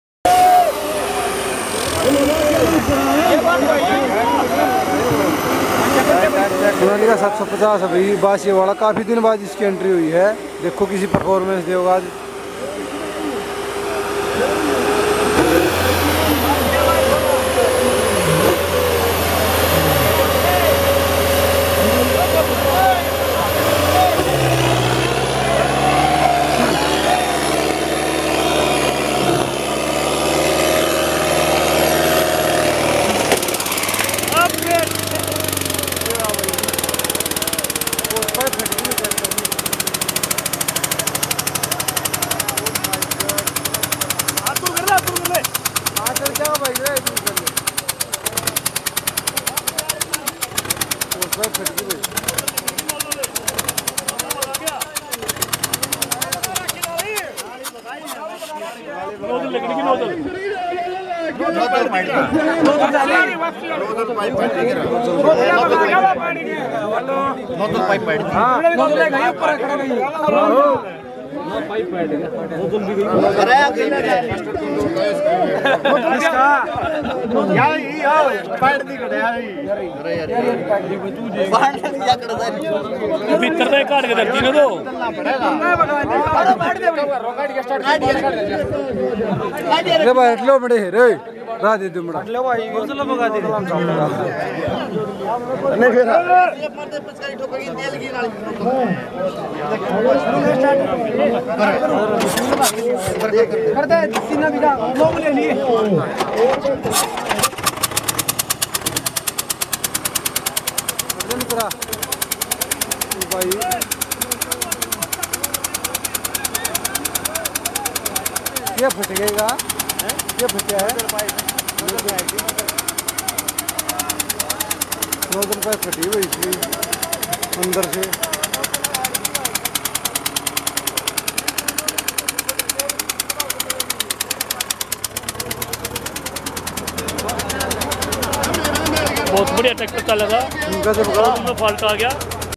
कुलैंट व नोजल पाईप भी फैंक दियाSonalika Di 750 Nojal pipe Failure in Gamri compition
Three Harrow competition Gamri Sonipat
Technical fault in sonalika tractor